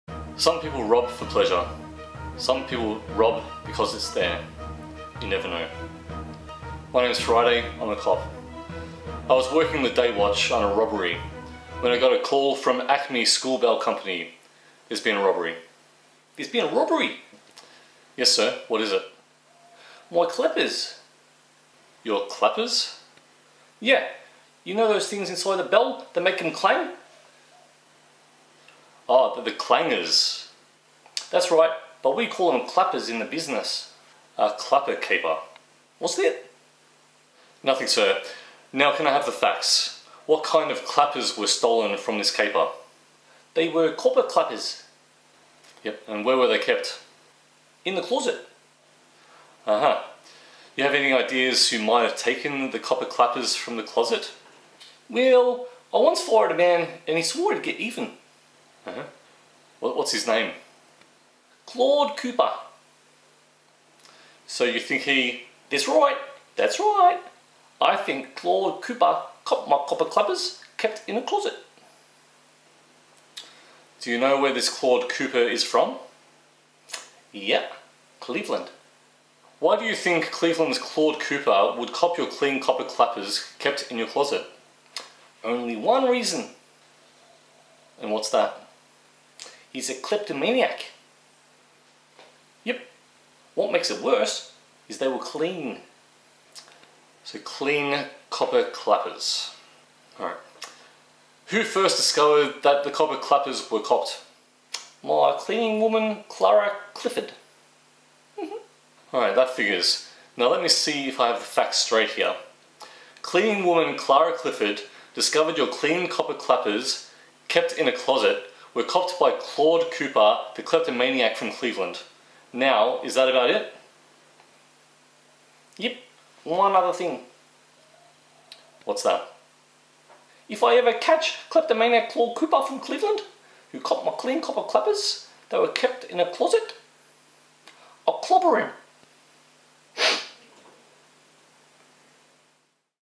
Copper Clappers
A short skit about a copper clapper caper